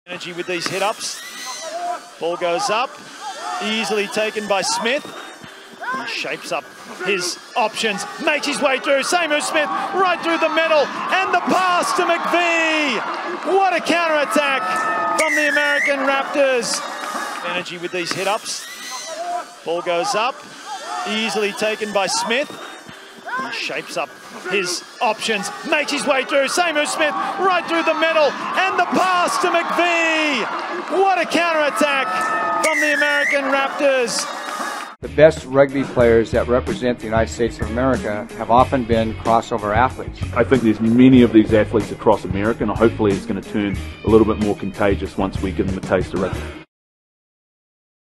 Ethan McVeigh Try From Seimou Smith Step Fend Offload AMERICAN RAPTORS vs. ST MARY'S COLLEGE 11.5.22
SEIMOU SMITH FOOTWORK FEND & ASSIST TO JASON MCVEIGH

Hive  - @rugbyfactorytv